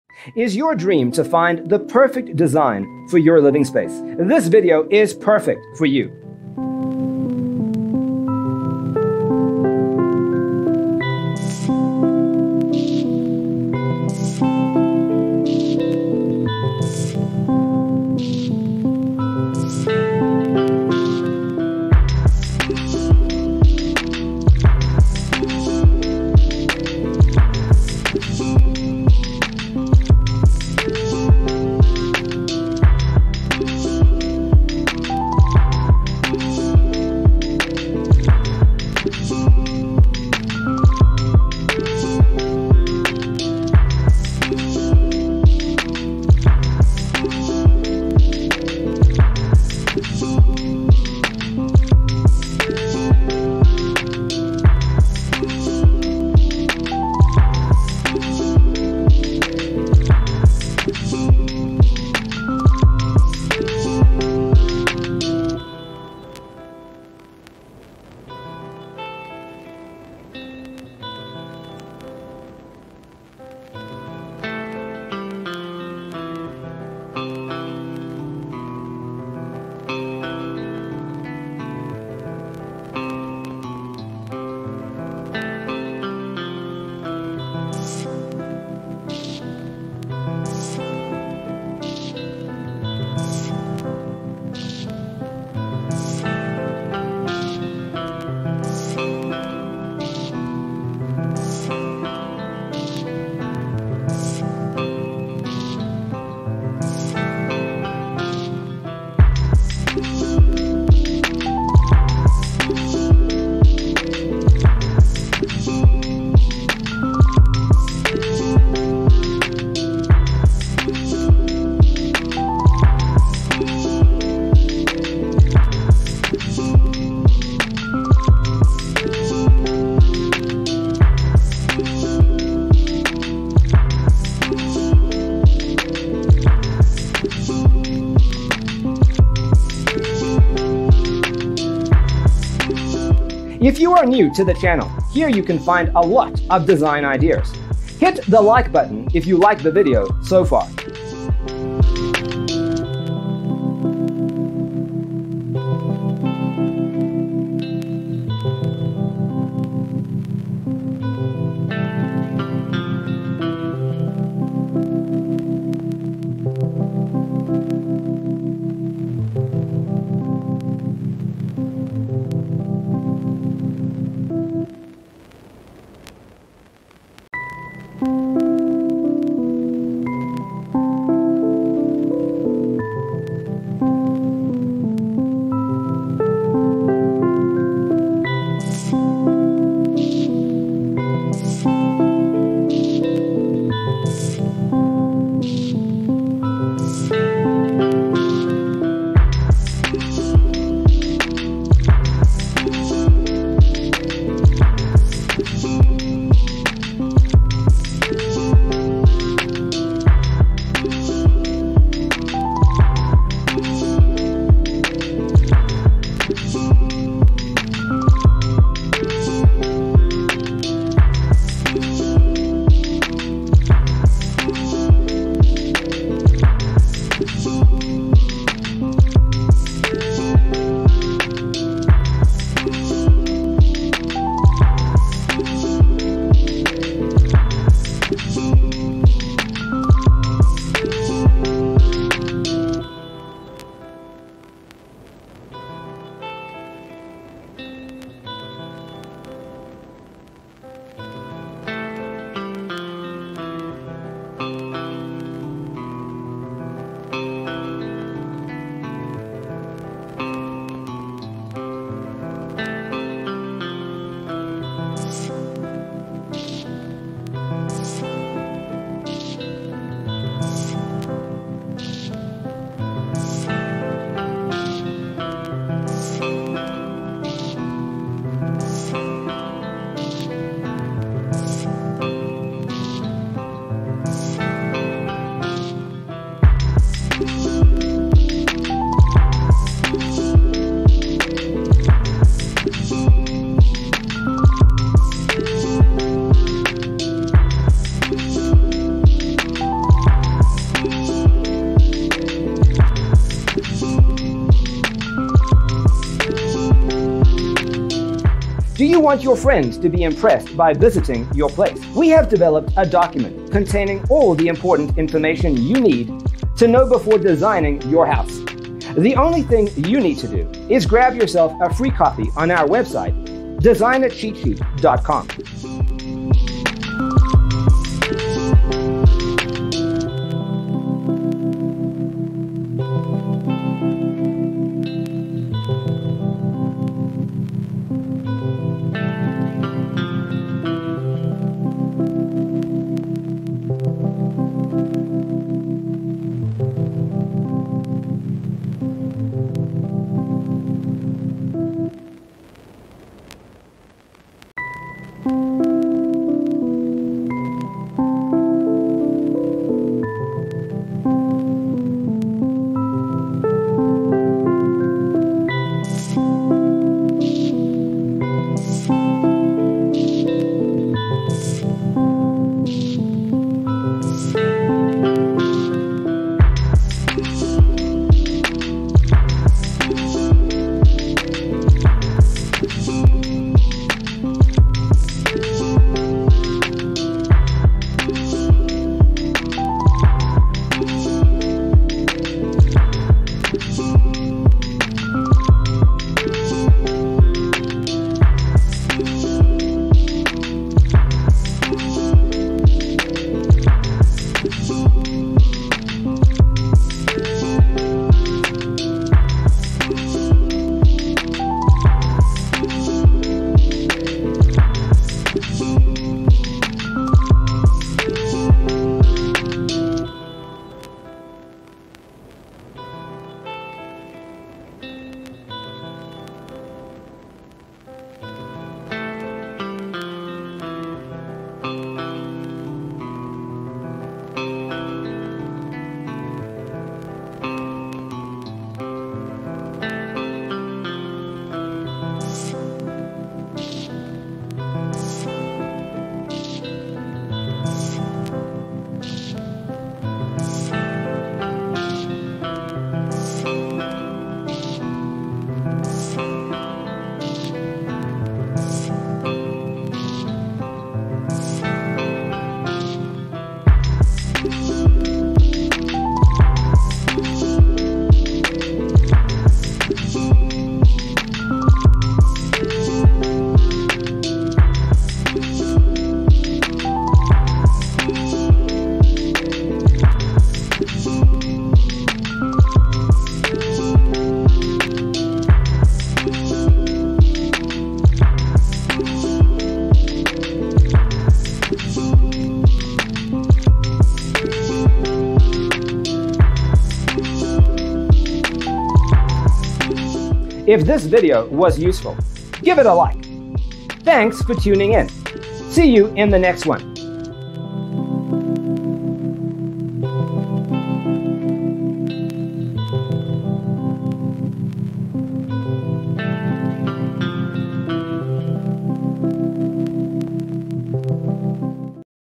75 Yellow Open Concept Living Room Design Ideas You'll Love 🔴
Browse yellow living room decorating ideas and furniture layouts. Discover design inspiration from a variety of yellow living rooms, including color, decor and storage options.

0:00:00 - Intro
0:00:12 - Yellow Open Concept Living Room Design Ideas You'll Love: Inspirations 1 to 25
0:03:01 - Yellow Open Concept Living Room Design Ideas You'll Love: Inspirations 26 to 50
0:06:02 - Yellow Open Concept Living Room Design Ideas You'll Love: Inspirations 51 to 75